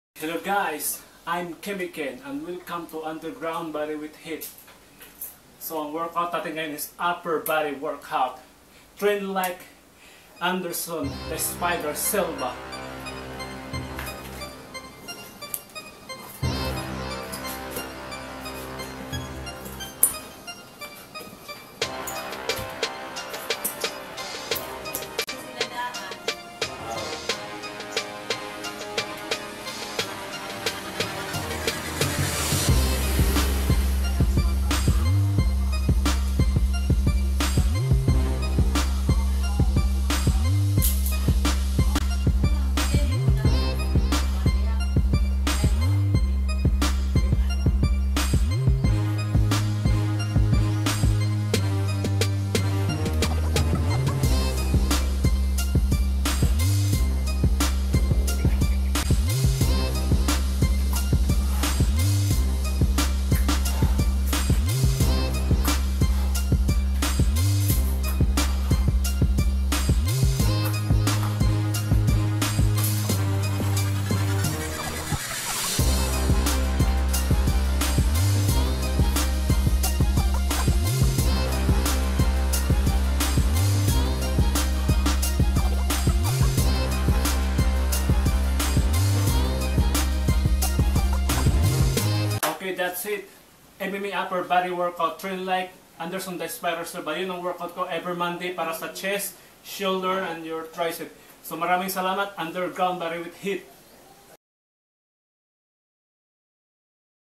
MMA Bodyweight Upper Body Workout - Underground Bodyweight HIIT
Upper Body Bodyweight Workout You Can Do With No Equipment!
Build Serious Muscle with These Upper Body Bodyweight Workout That Can Be Done Anywhere!
And Get in shape and feel great about your body! Gain strength, muscles and lose body fat!
MMA bodyweight upper body workout to build your arms, shoulders
Chest,Triceps Train like Anderson "The Spider" Silva UFC
1. Hanuman Dand Push Up 8-10-12 Reps rest 45 seconds 3 sets
2.Dive Down Push Up 8-10-12 Reps rest 45 seconds 3 sets
3.Pike Push Up 8-10-12 Reps rest 45 seconds 3 sets
4.Dive Down Triangle Push Up 8-10-12 Reps rest 45 seconds 3 sets
5.Kick Push Up 8-10-12 Reps rest 45 seconds 3 sets
6.Skull Crushers 8-10-12 Reps rest 45 seconds 3 sets